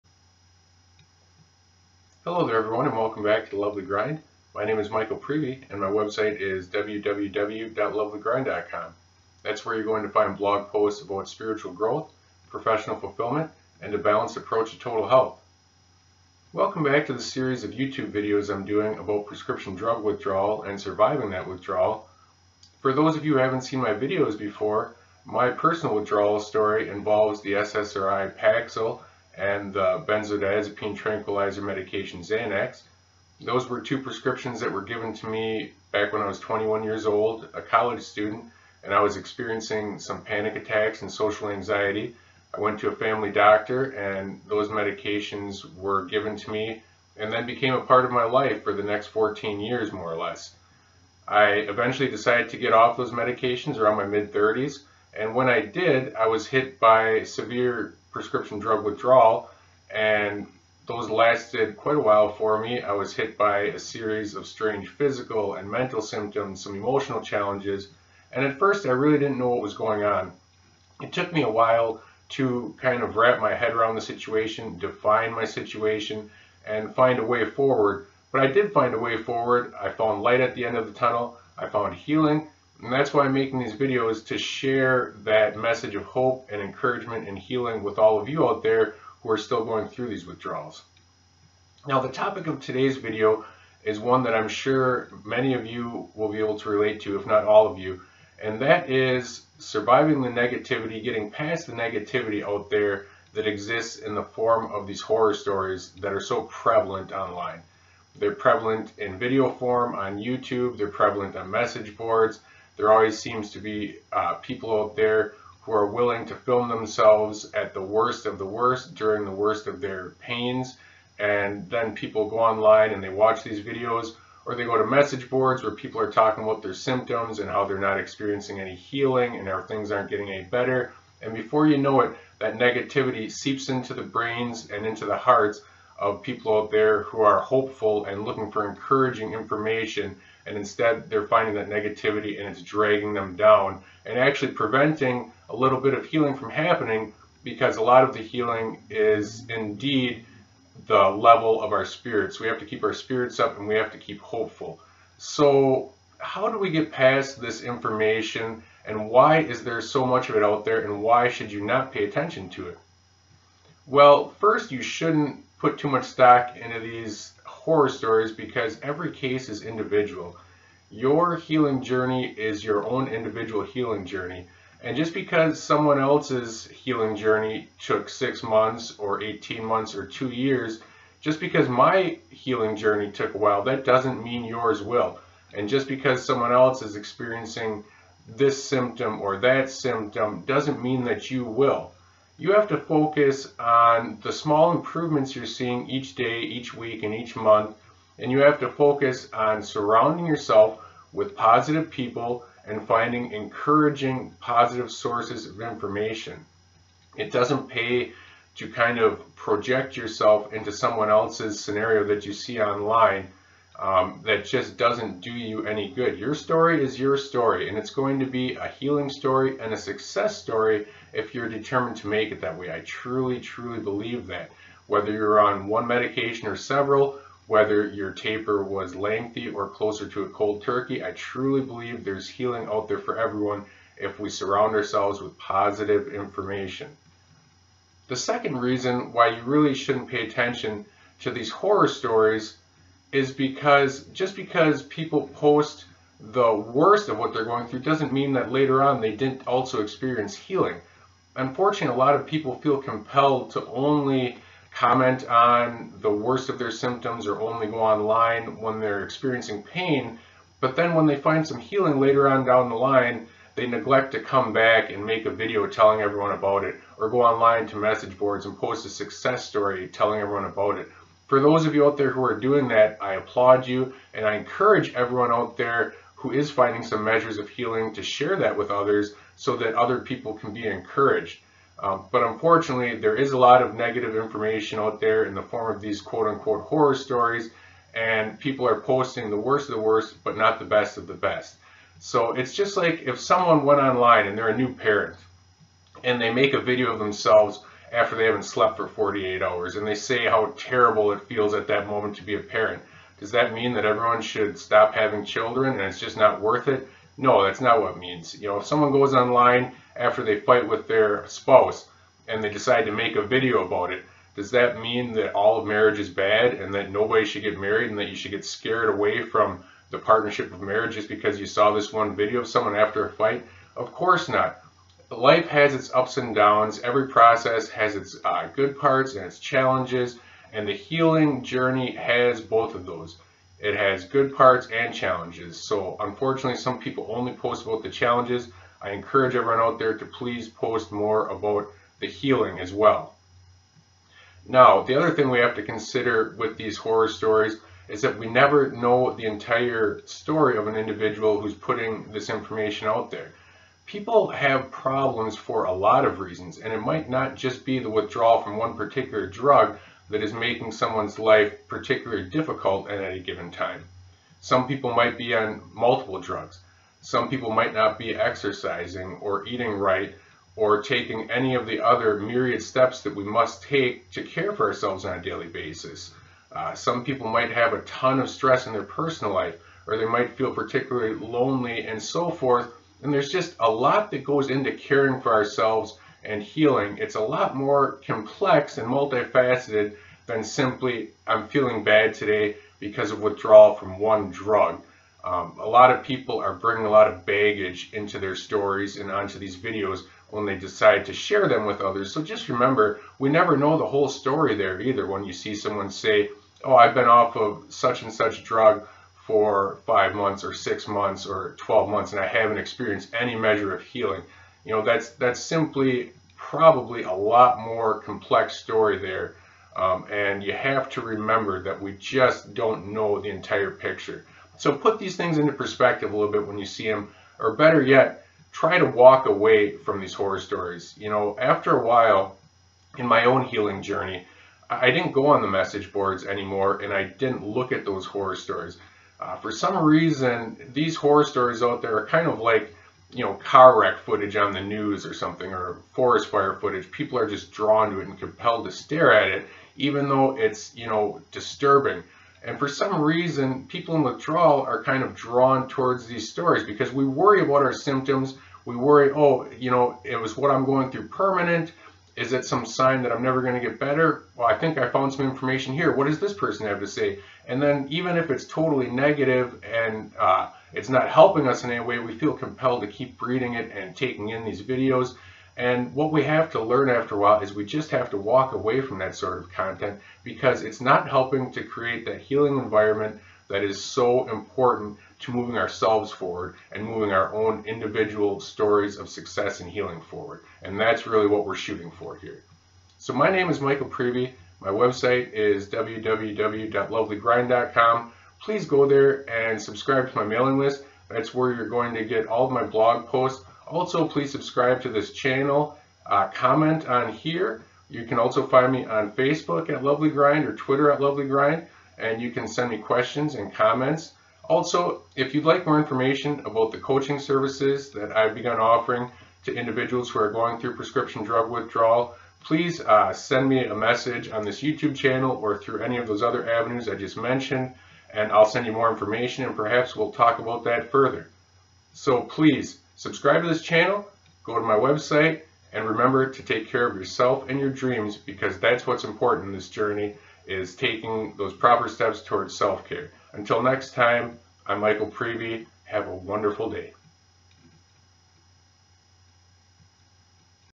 Withdrawal: Ignore the Horror Stories
When we begin experiencing benzodiazepine or antidepressant withdrawal, we are desperate to find hope and solid information. Unfortunately, we often find "horror stories" instead. In order to progress in our own healing journey, we must put these negative videos and posts into context, and eventually we must learn to ignore them altogether. for more of my positive and inspiring content.